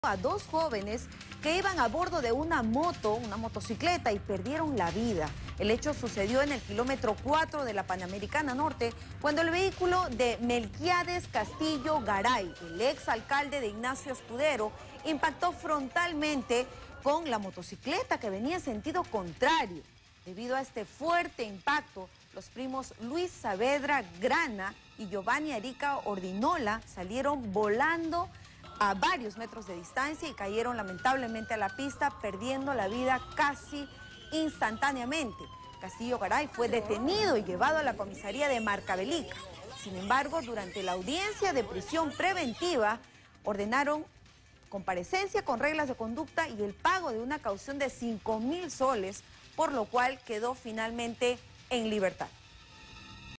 Piura: exalcalde provoca muerte de jóvenes en accidente y lo dejan libre
En un trágico accidente ocurrido en Piura, dos jóvenes perdieron la vida al ser arrollados por una camioneta en la Panamericana Norte, a la altura del kilómetro 4. Las cámaras de seguridad de la zona captaron el momento en que el vehículo, conducido por Melquiades Castillo Garay, exalcalde de Ignacio Escudero, impactó frontalmente con la motocicleta en la que viajaban los primos Luis Saavedra Granda y Giovani Arica Ordinola.

El fuerte impacto lanzó a las víctimas varios metros, cayendo en la pista y perdiendo la vida casi de manera instantánea. Tras el accidente, Castillo Garay fue detenido y llevado a la Comisaría de Marcavelica. Sin embargo, durante la audiencia de presión preventiva, el juez le otorgó la comparecencia con reglas de conducta y el pago de una caución de 5 mil soles, lo que permitió que el exalcalde quedara en libertad mientras continúa la investigación.

El suceso ha conmocionado a la comunidad de Piura, mientras se espera que las autoridades determinen las causas del accidente y las responsabilidades legales correspondientes en este trágico incidente.